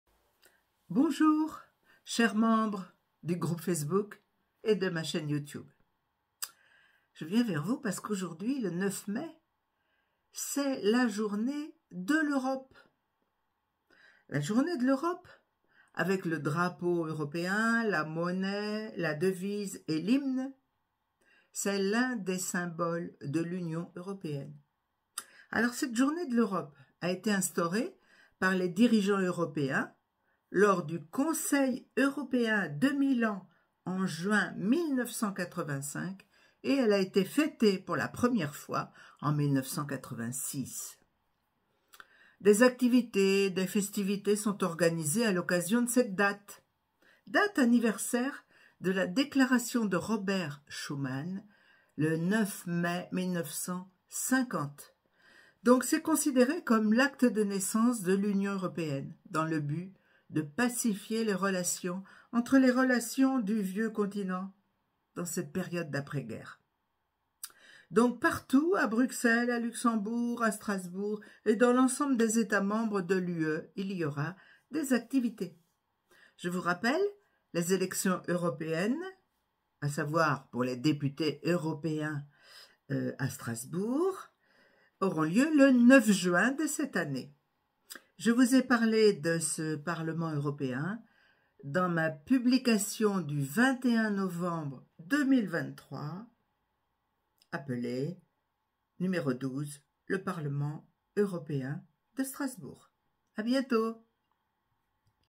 La fête de l'Europe, le 9 mai
La journée de l’Europe (9 mai)
La Journée de l’Europe est, avec le drapeau, la monnaie, la devise et l’hymne européens, l’un des symboles de l’Union européenne. La Journée de l’Europe a été instaurée par les dirigeants européens lors du Conseil européen de Milan en juin 1985 et fêtée la première fois en 1986.
Activités et festivités sont organisées à l’occasion de cette date anniversaire de la Déclaration Robert Schuman du 9 mai 1950, considérée comme “l’acte de naissance” de l’Union dans le but de pacifier les relations entre les nations du Vieux Continent dans la période d’après-guerre. Ce sera partout à Bruxelles, à Luxembourg, à Strasbourg et dans l’ensemble des États membres de l’UE.
Rappel : Les élections européennes (pour les députés européens au parlement de Strasbourg) auront lieu le 9 juin.
Voir la publication parue le 21/11/2023 (12 Le parlement européen (Strasbourg))